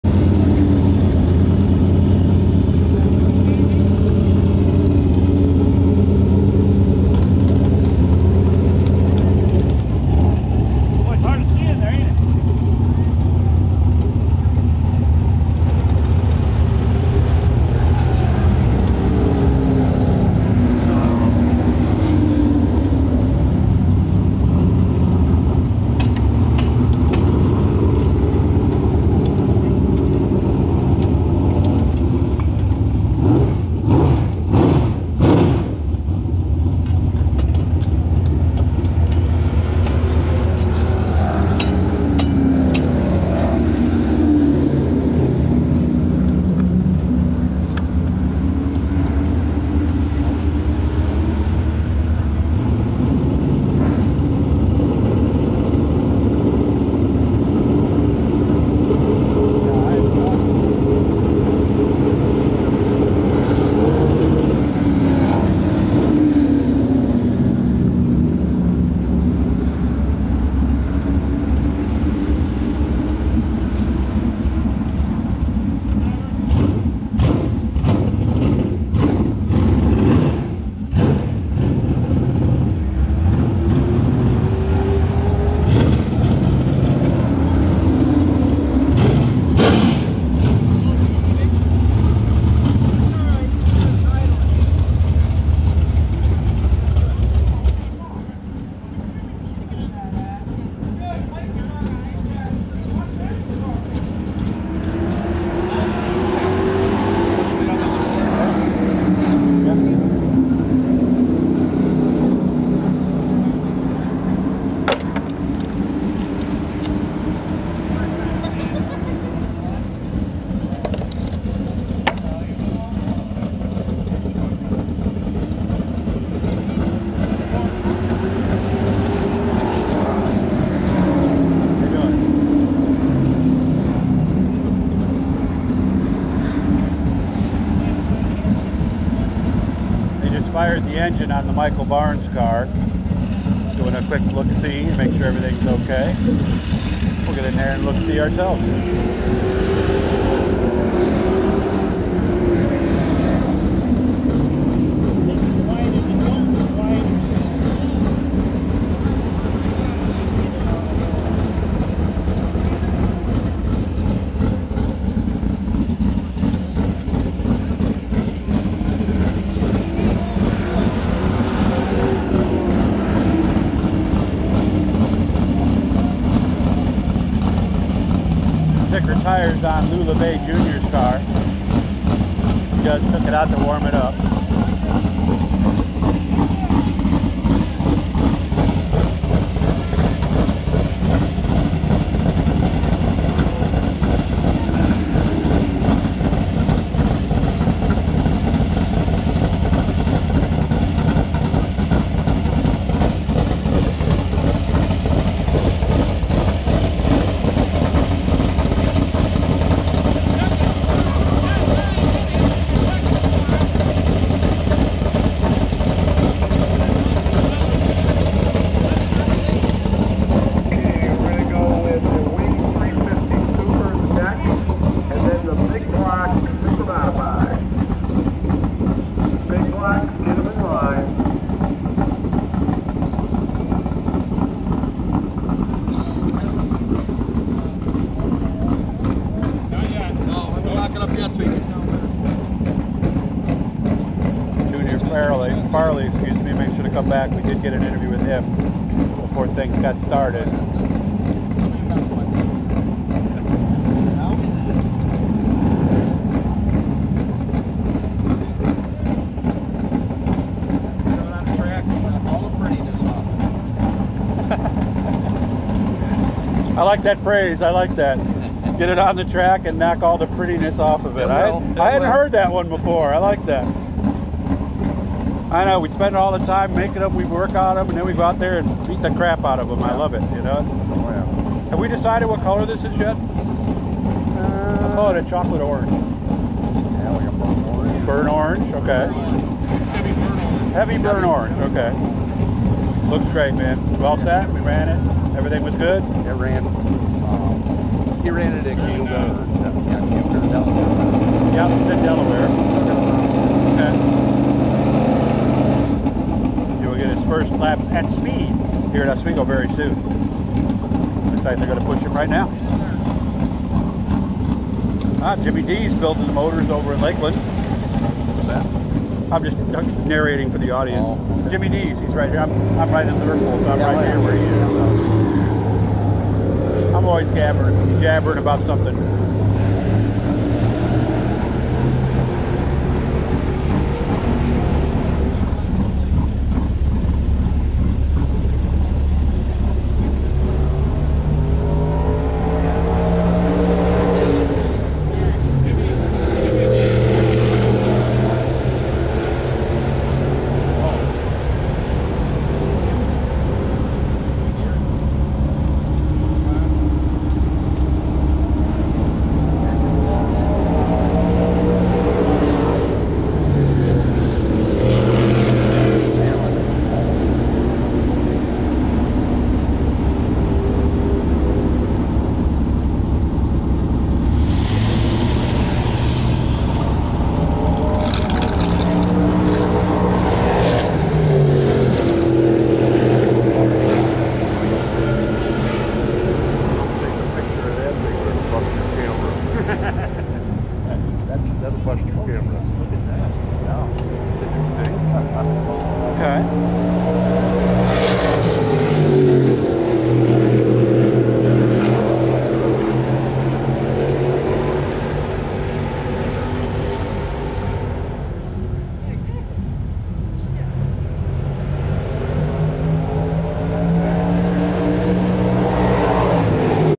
Racecars getting ready and heading out at Oswego Speedway May 24 2024
Watch some racecars in the line and ready to go out at Oswego Speedway's Fast Friday practice. Something for you guys to watch as you wait out the rain!